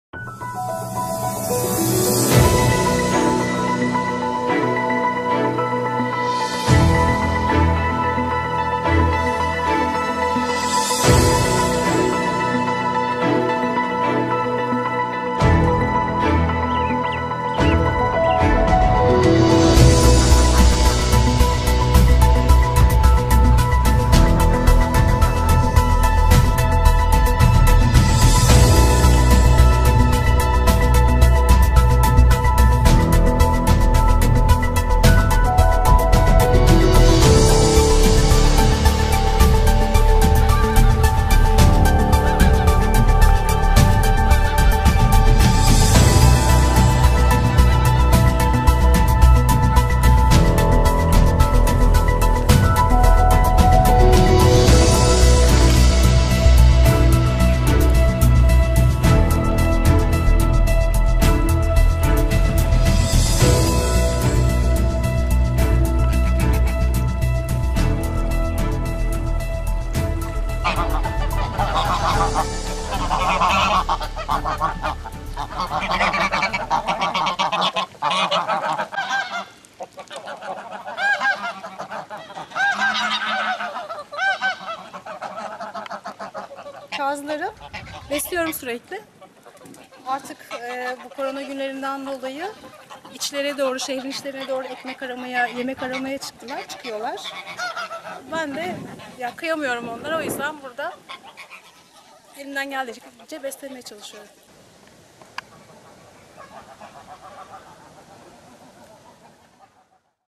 Akyaka'da Azmağın sembolleri aç kaldı
Muğla’nın dünyaca ünlü Akyaka mahallesindeki Kadın Azmağı kenarındaki bulunan restoranların Kovid-19 tedbirleri kapsamında geçici olarak kapatılması sonrası azmakta yaşayan yüzlerce kaz ve ördek yem bulmak için mahalle içlerine gitmek zorunda kalıyor.